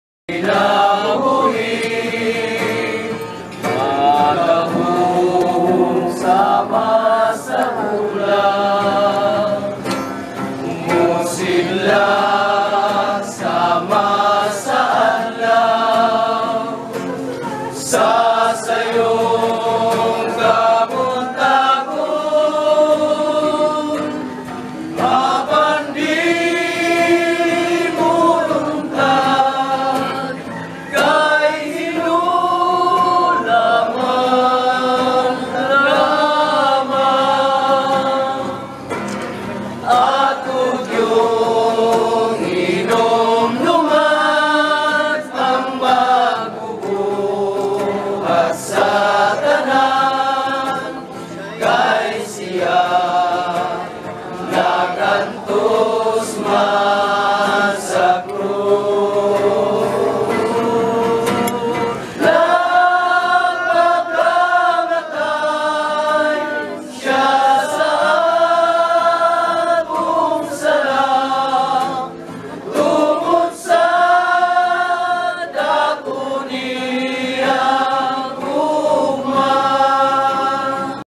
Inmates Worship
Inmates in Philipines worshipping.